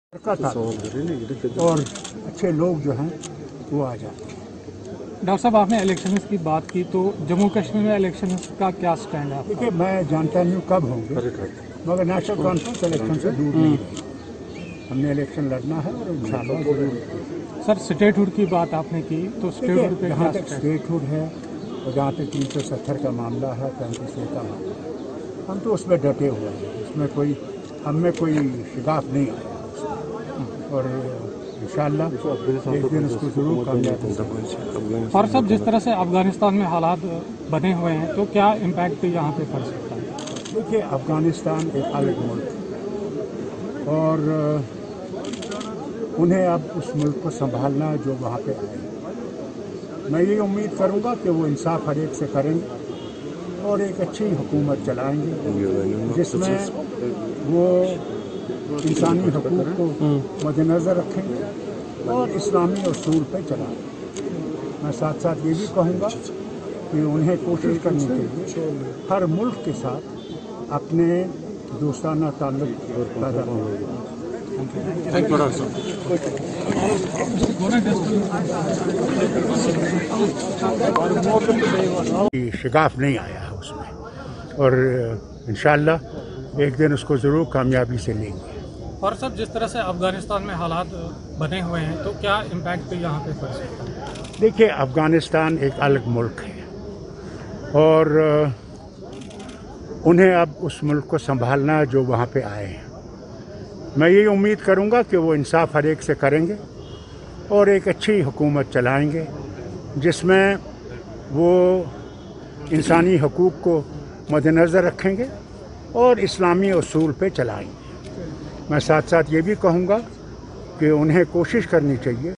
LIVE: Dr. Farooq Abdullah, former J&K CM backs the Taliban government in Afghanistan
LIVE: Dr. Farooq Abdullah, former J&K CM backs the Taliban government in Afghanistan.” I hope taliban governs the country well, keeping in view the human rights and doing justice to all and run the country on Islamic rules”, He said